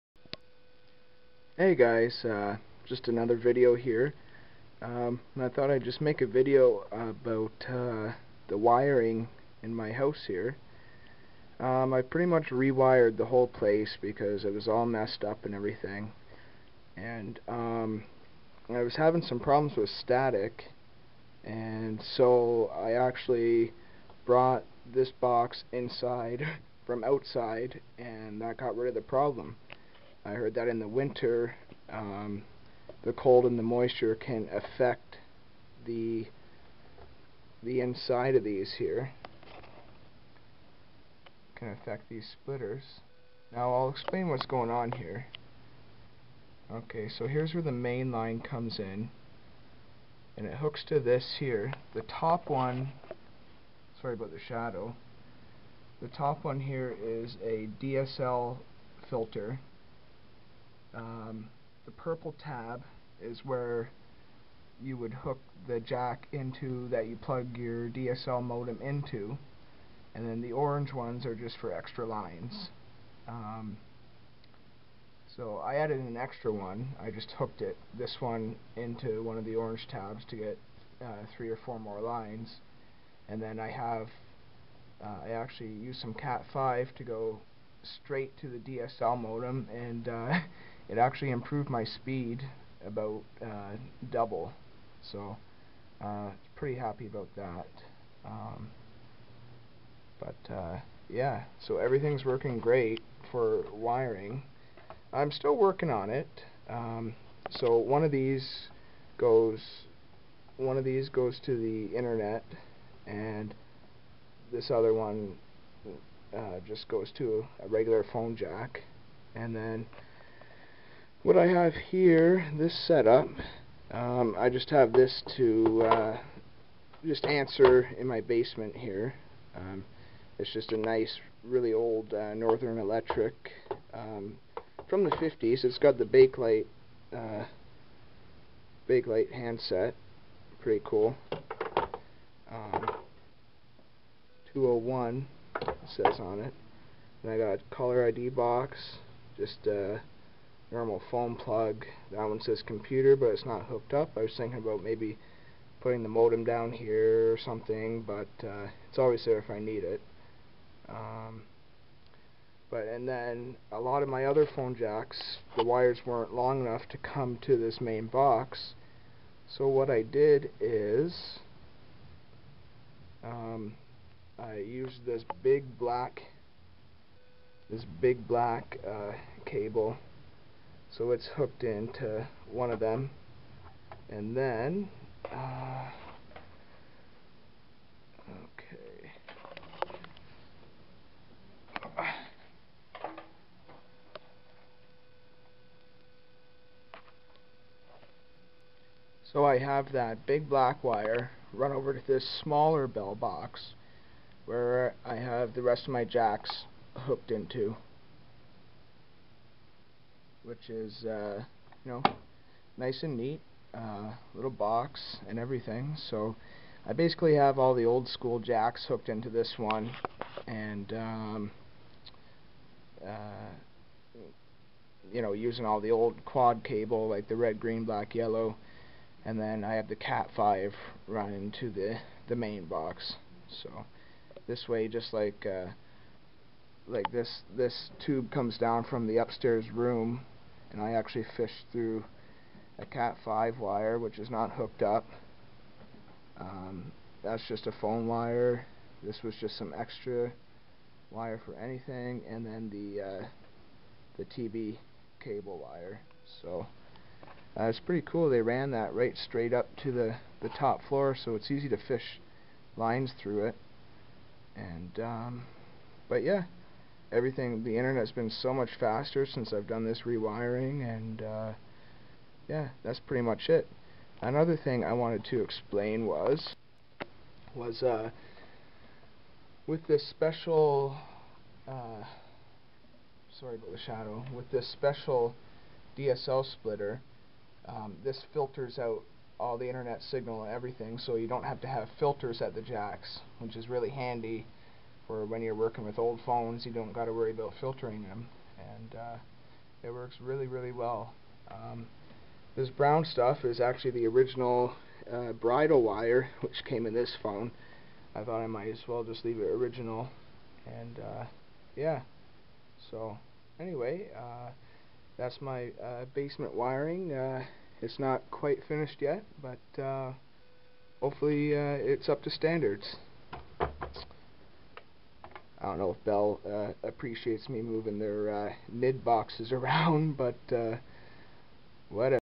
Basement bell phone wiring
Just showing my phone and internet wiring in my basement.